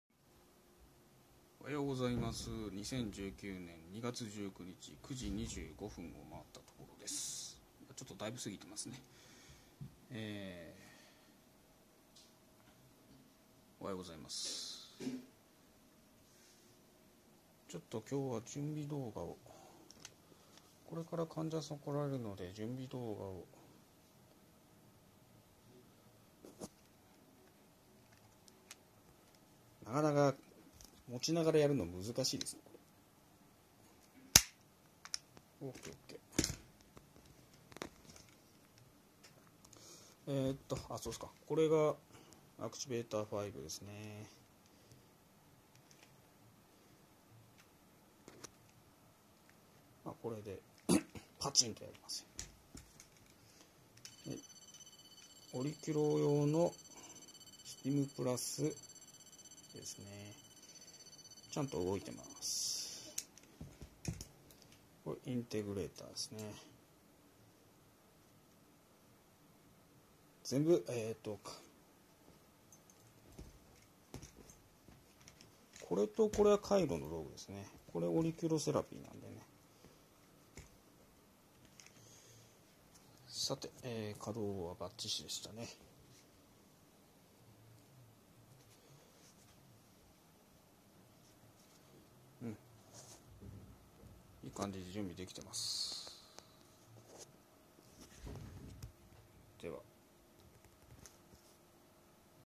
カイロプラクティックケア前のツールチェック
カイロプラクティックケア前にツールがちゃんと動くか確認しています。

ちなみにアクティベータ5とインテグレーターを両方持っている治療家は結構少ないと思います。

腰痛や肩こりにはいい感じでケアできますよ。

撮影技術をもう少し向上させたいですね。